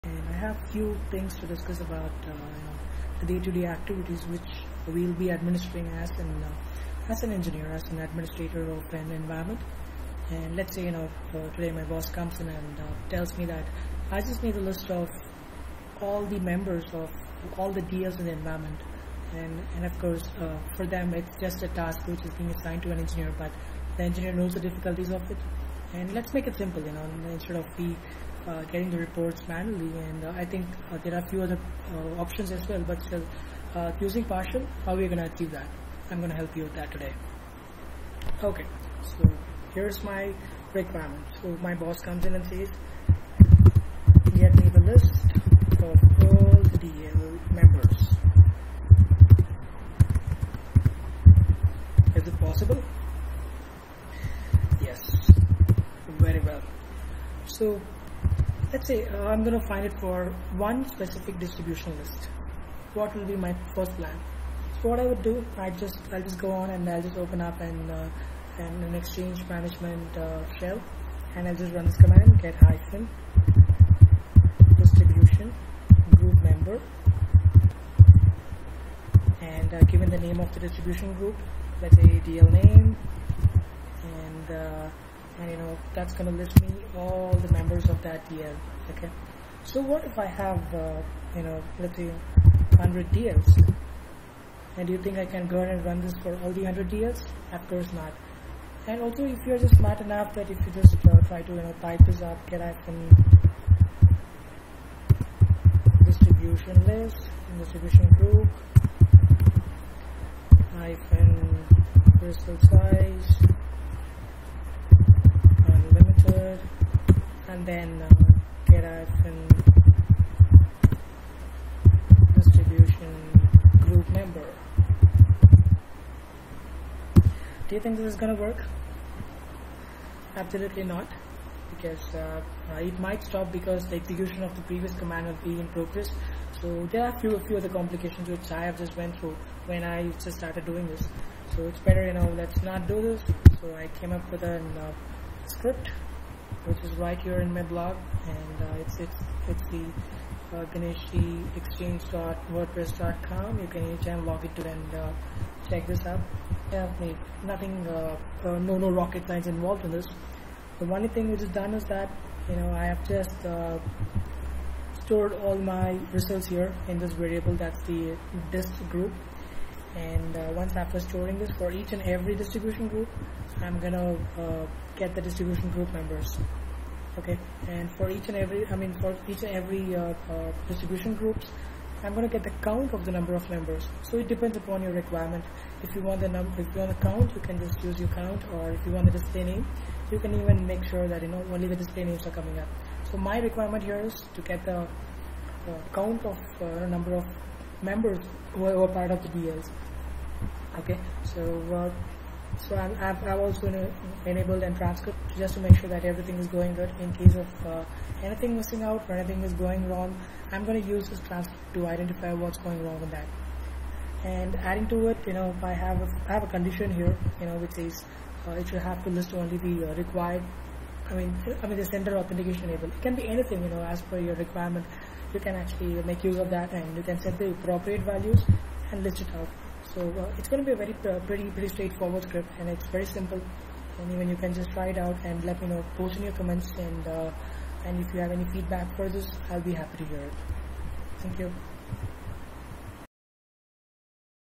Exchange Training 1 : Script - Exchange 2010 - Get the Distribution Group Members
Script to Get the member counts in all DL which are enabled for receiving external emails

"Get-distributiongroup | get-distributiongroupmember "

this command will execute fine but will not have the appropriate DL name and its members.

Watch the video and let me know if you have any questions